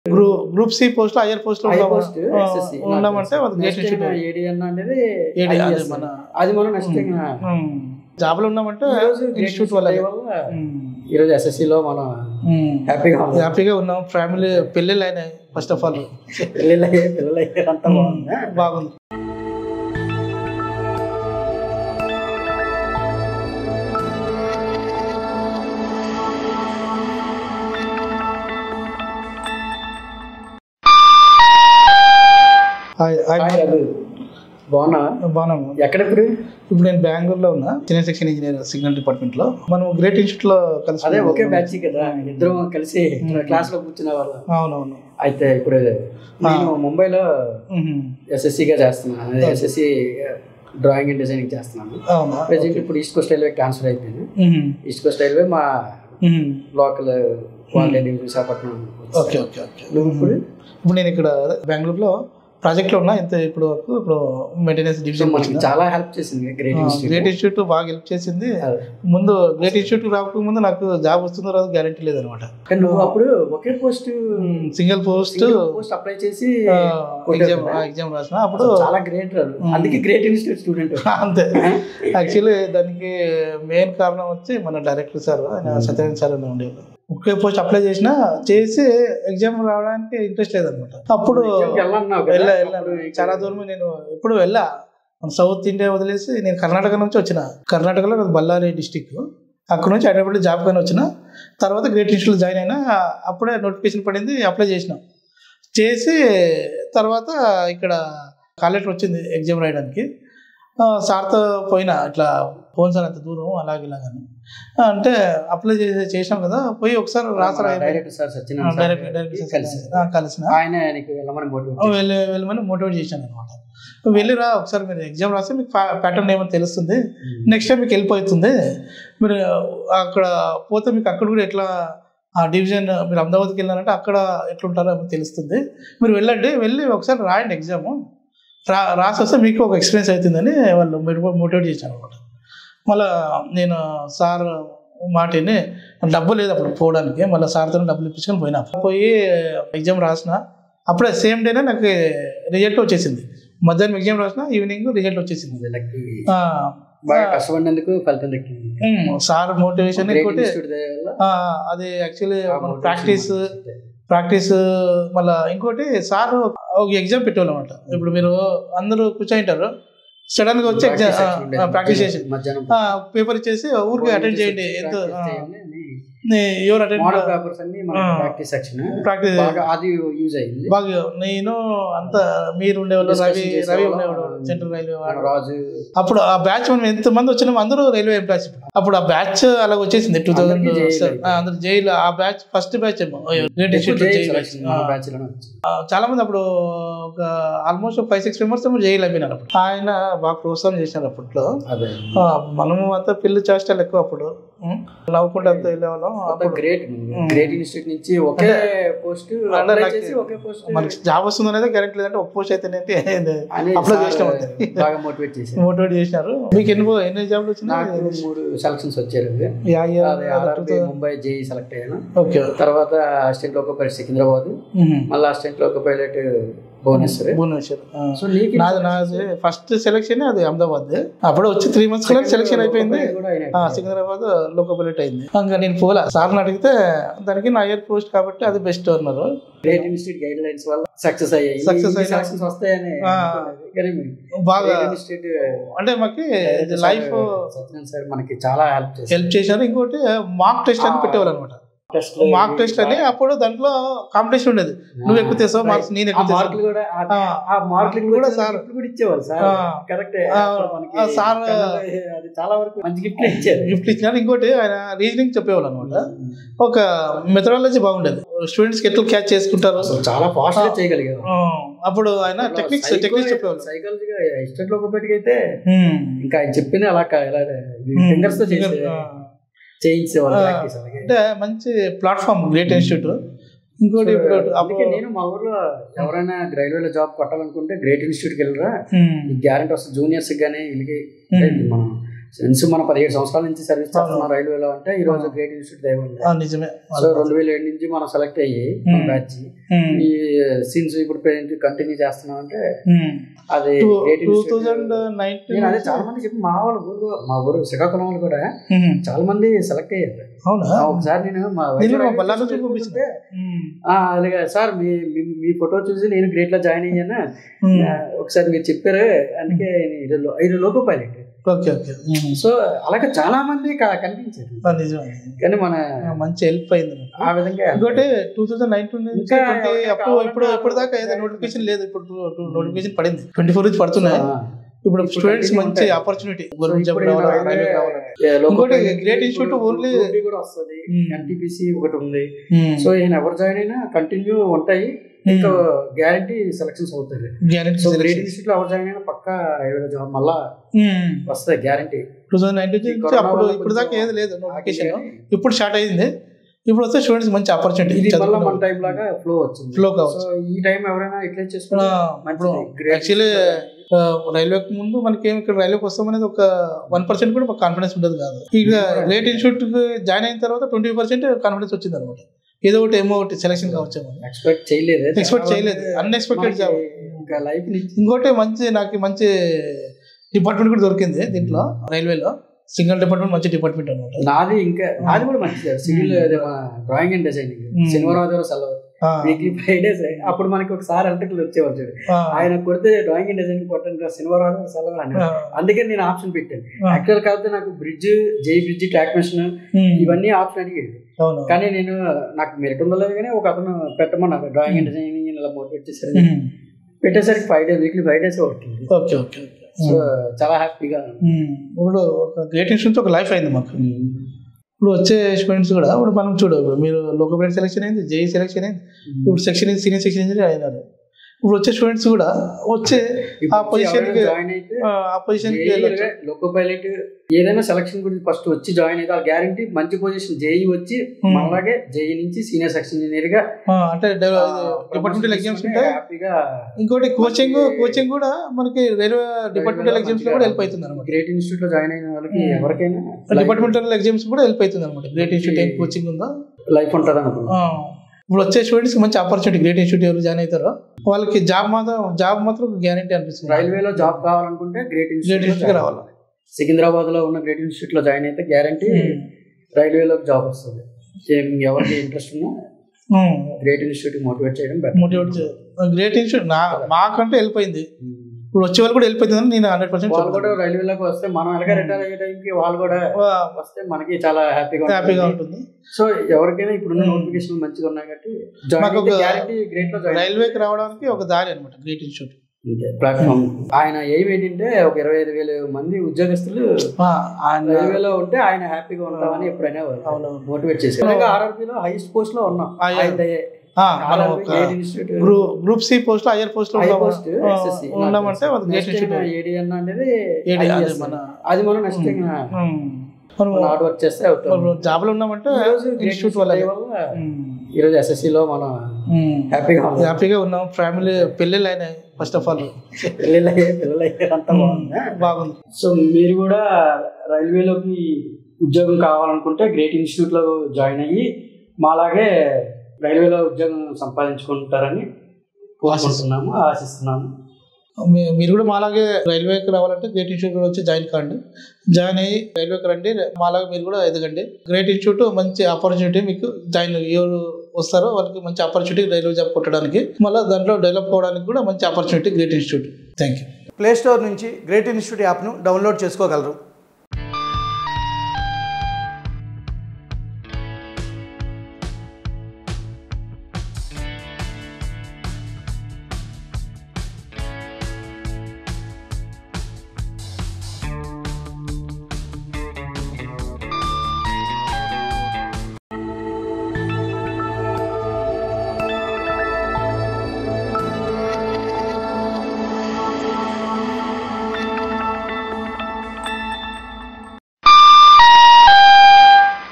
నోటిఫికేషన్లో ప్రకటించిన కేవలం ఒకే ఒక్క JE పోస్టును సాధించిన గ్రేట్ విద్యార్థి
GREAT INSTITUTE - RRB - The best coaching center for the railway jobs , Padmarao Nagar, Secundrabad - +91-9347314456, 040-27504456